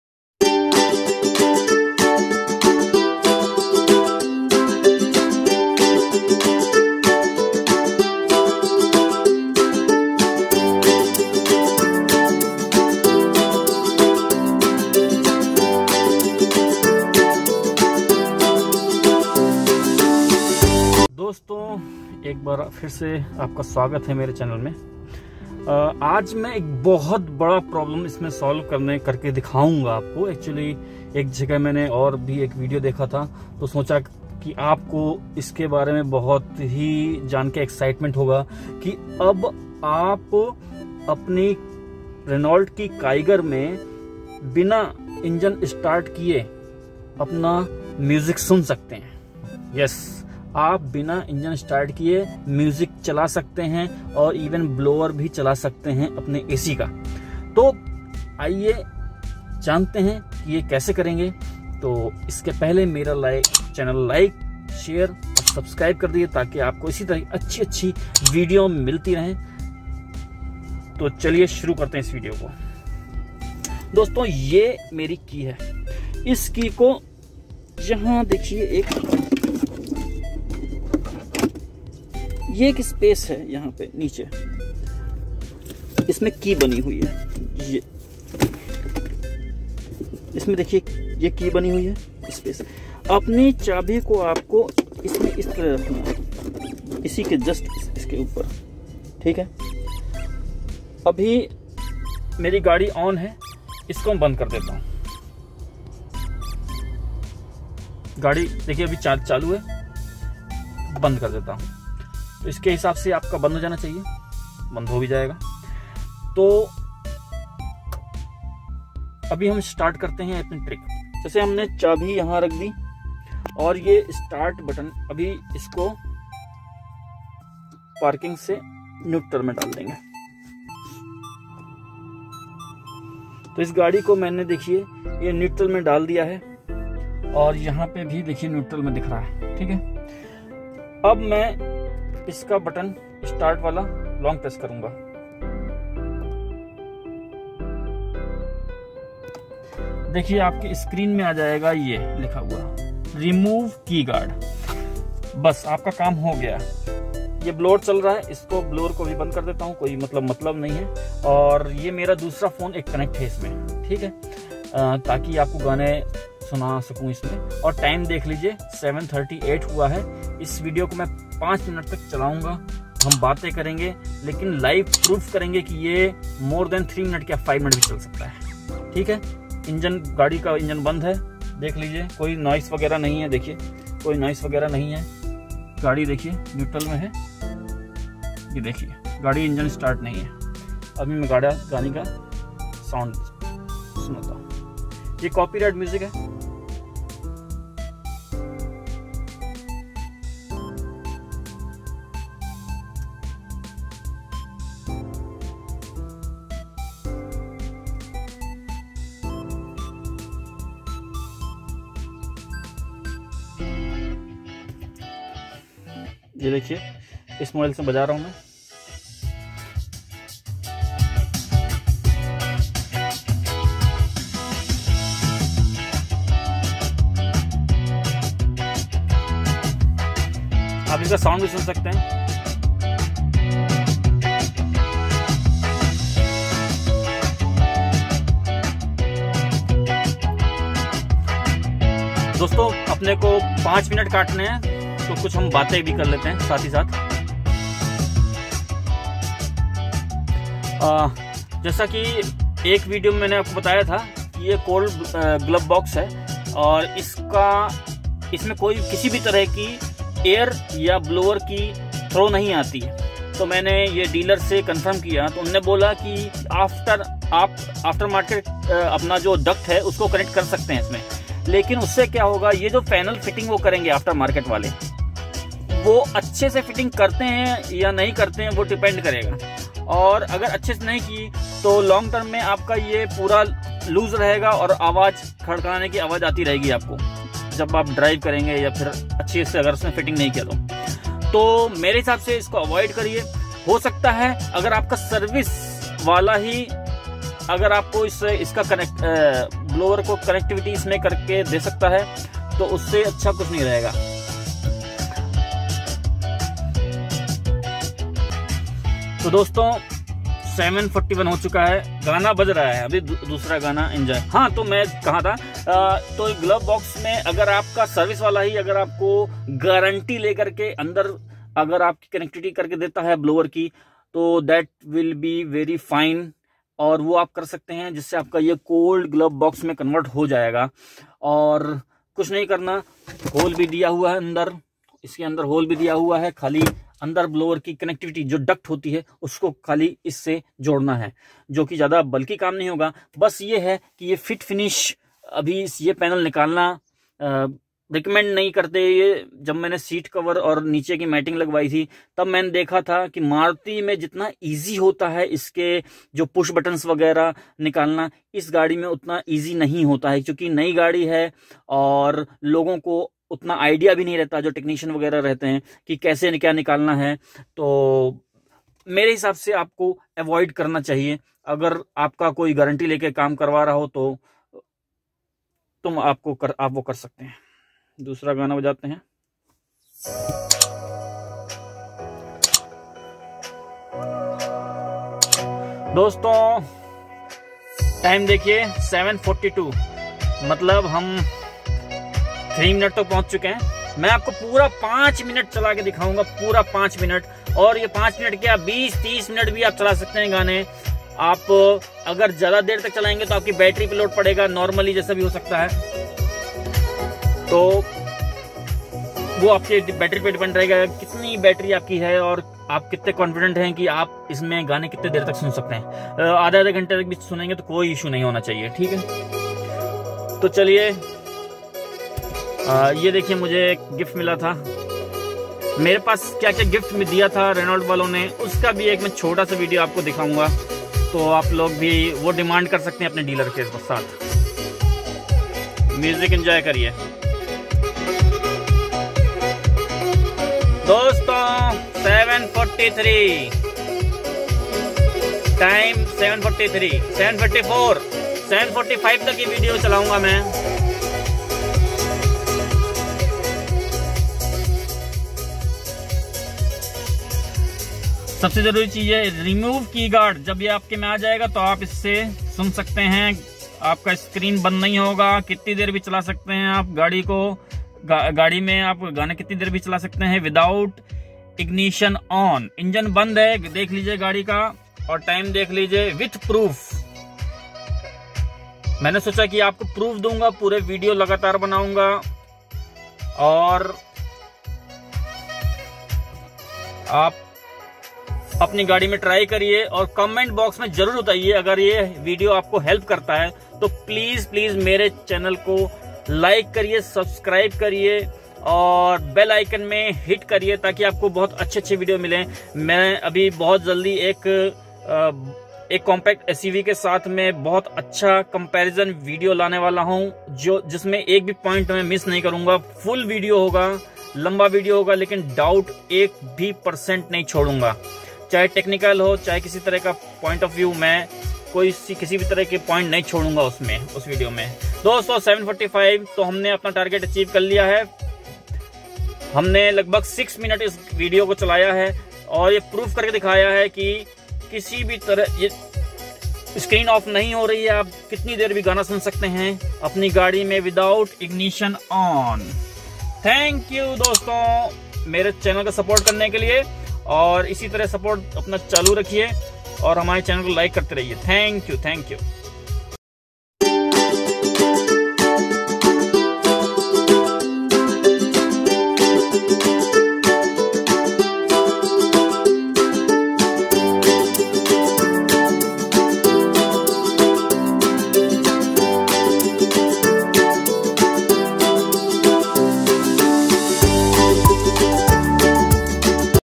Renault Kiger Music problem solution
Now you can play music without engine on. In case of Manual transmission You need to put your vehicle in gear .problem of music in vehicle engine Off solved for Kiger RXT/RXZ model and also Magnite XV and XV premium model .